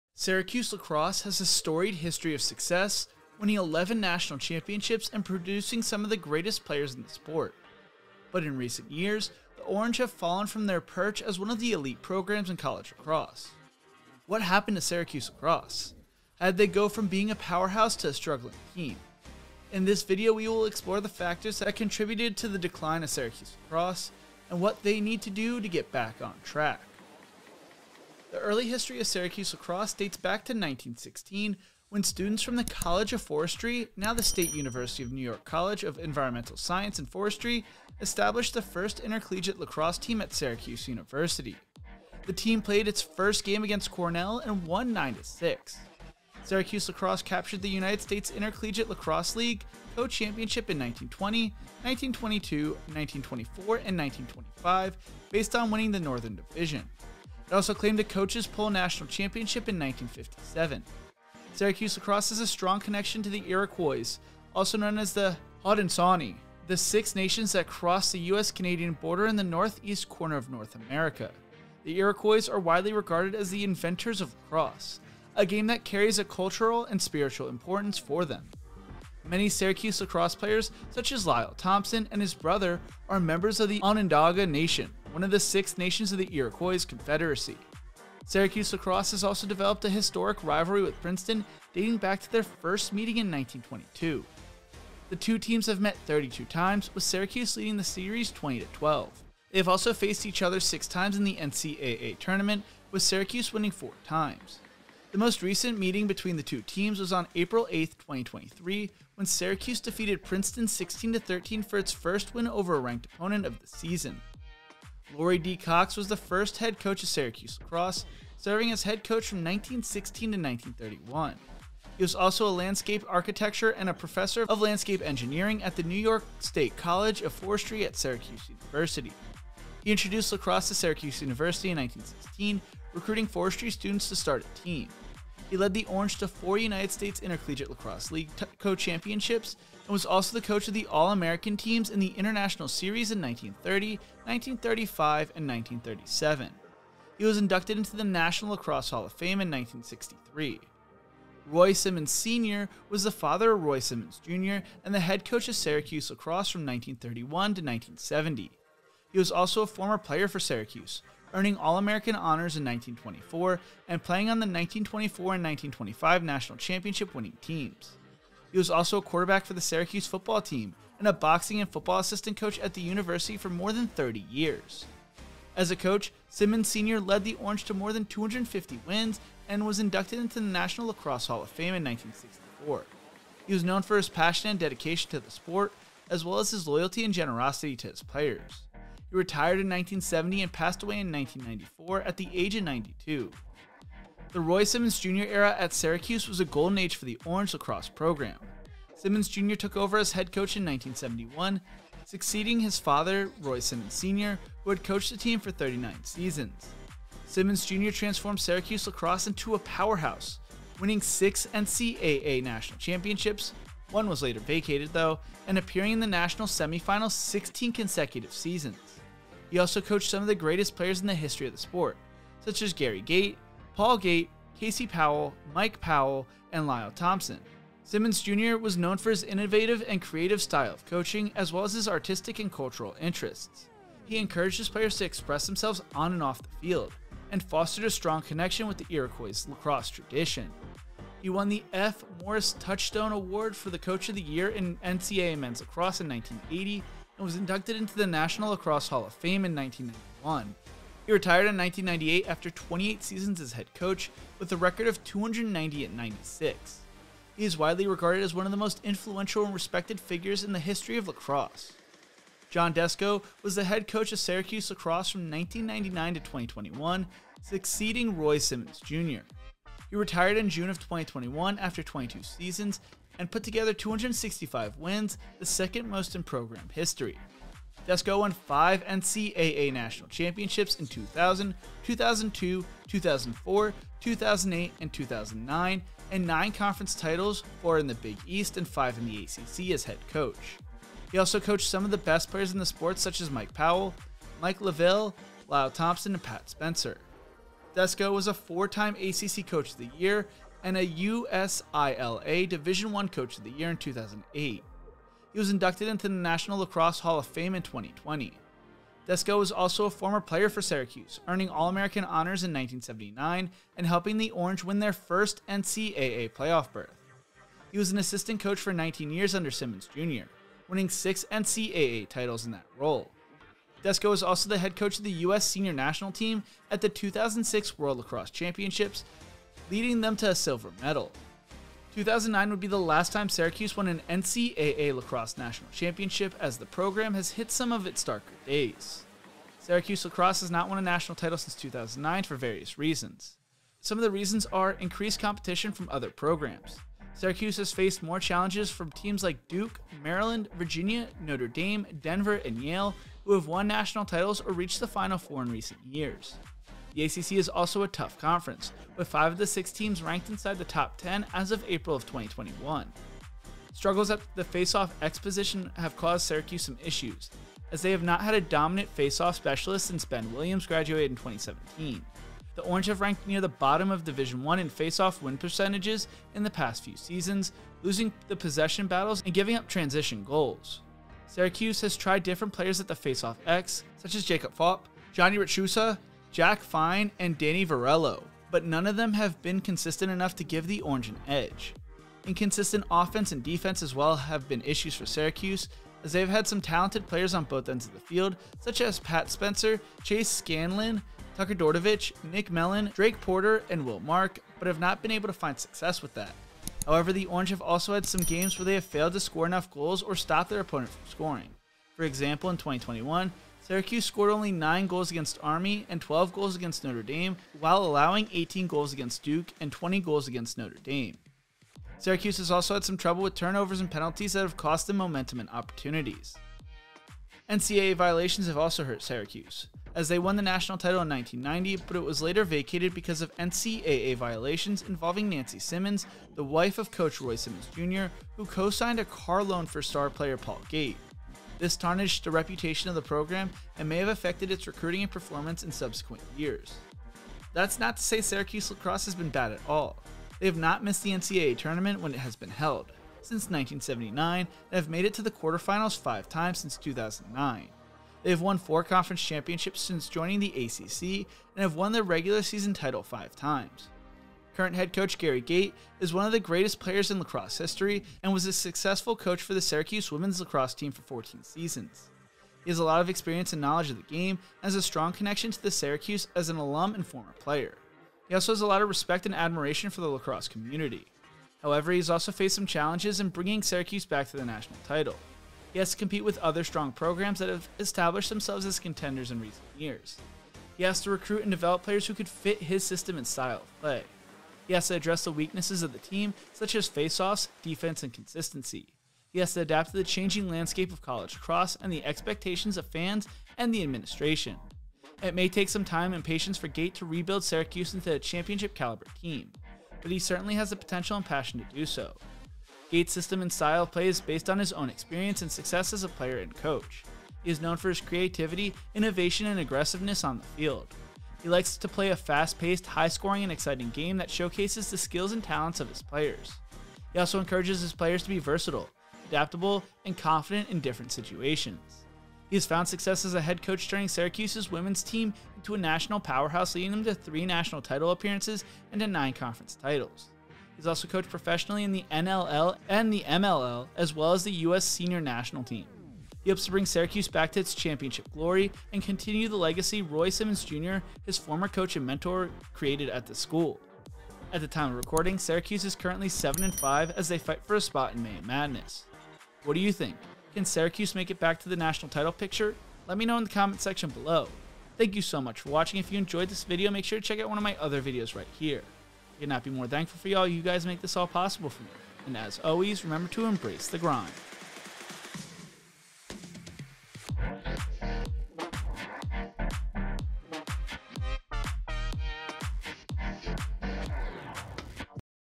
The Fall of a Powerhouse: The Story of Syracuse Lacrosse
Syracuse lacrosse has a storied history of success, winning 11 national championships and producing some of the greatest players in the sport. But in recent years, the Orange have fallen from their perch as one of the elite programs in college lacrosse. What happened to Syracuse lacrosse? How did they go from being a powerhouse to a struggling team? In this video, we will explore the factors that contributed to the decline of Syracuse lacrosse, and what they need to do to get back on track. We will look at the history and legacy of Syracuse lacrosse, the challenges and changes they faced in the modern era, and the hopes and expectations for the future. If you are a fan of lacrosse or sports history, you don’t want to miss this video. Watch now and learn more about the fall of a powerhouse: the story of Syracuse lacrosse.